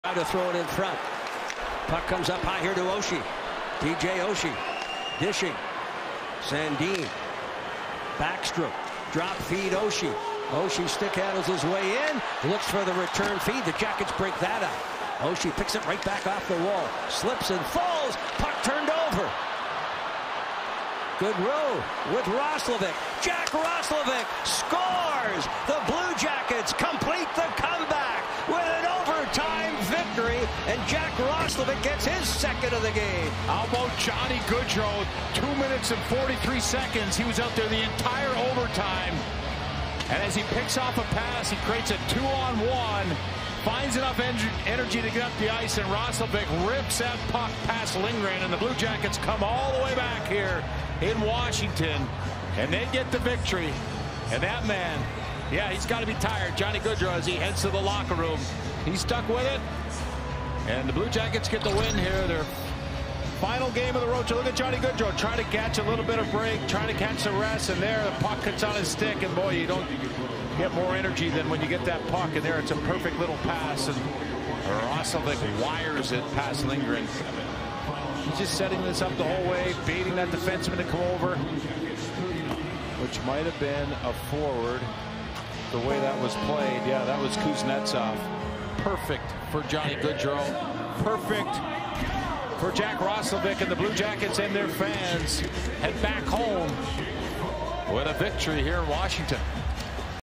Jack Roslovic OT Winner
clip from Columbus Blue Jackets @ Washington Capitals on March 21st, 2023